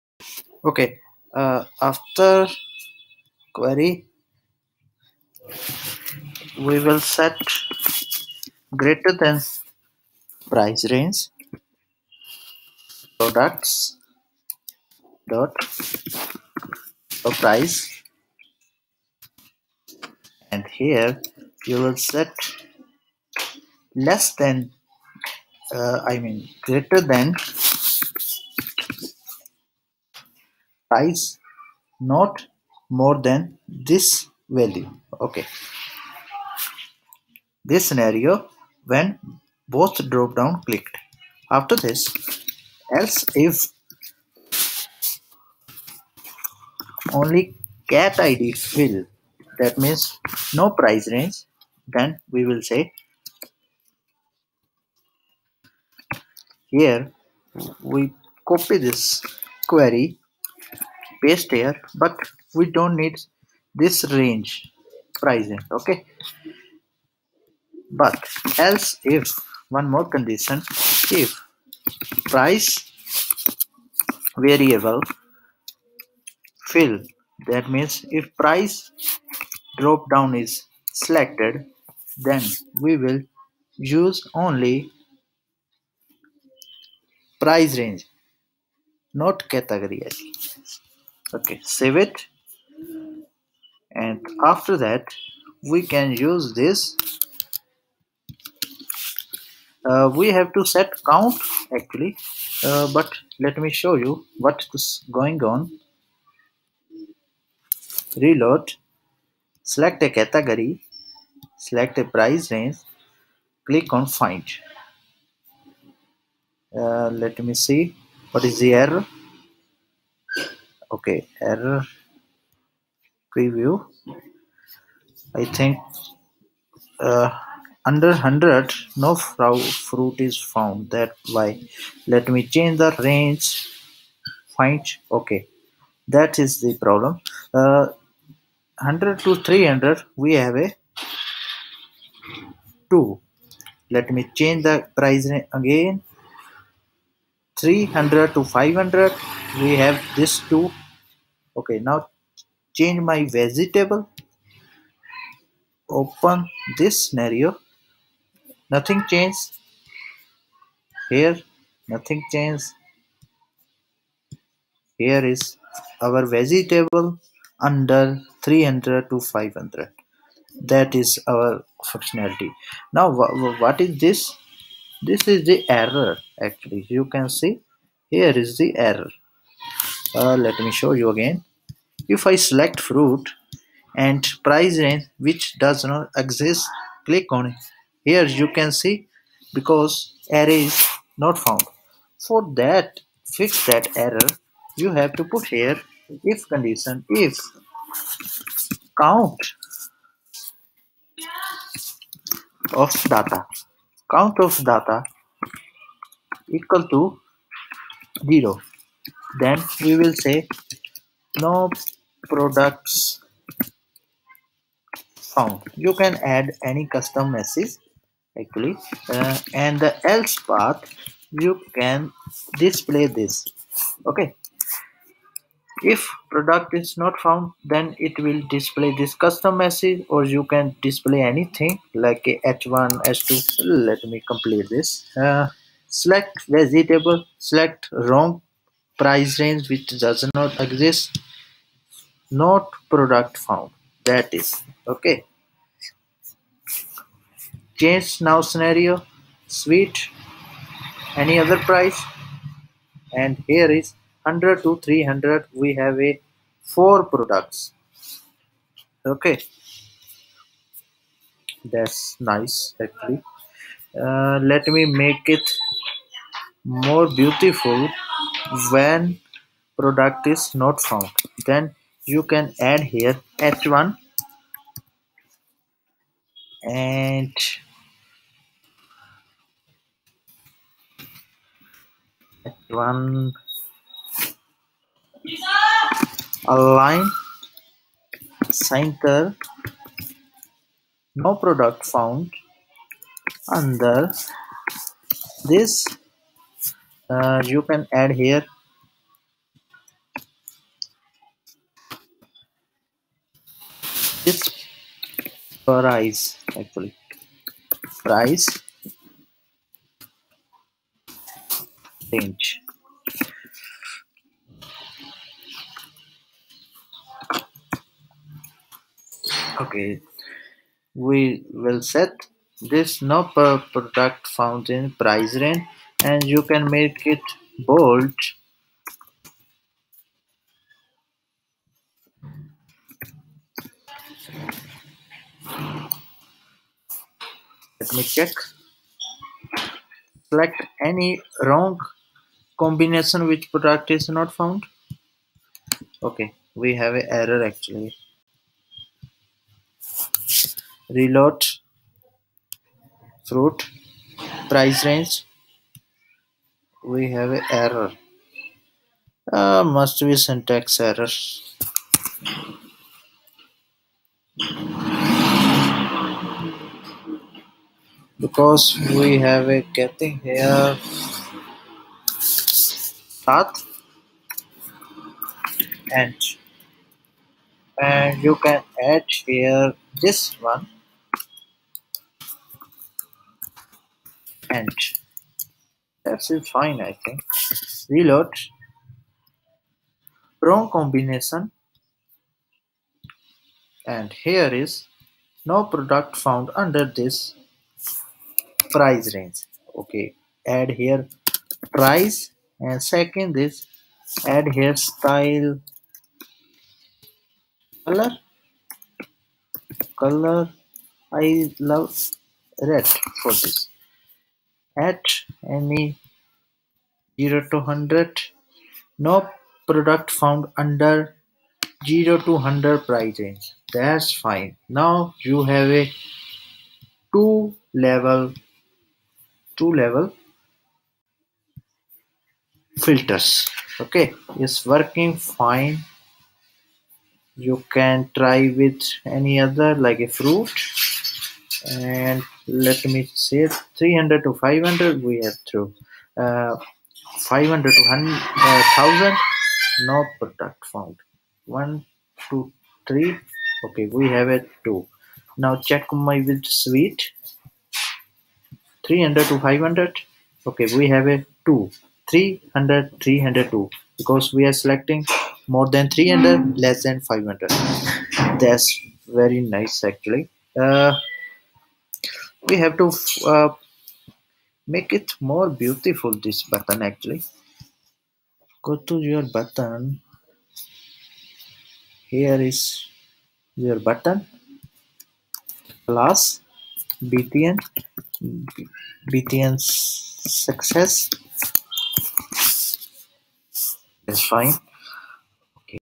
#15 Price Range Filter | Dropdown Ajax filter | Products Sorting | E-Commerce website with Laravel
In this video we will set two drop-downs for price range and categories with Ajax. our product can have multiple attributes such as price range, brand and categories.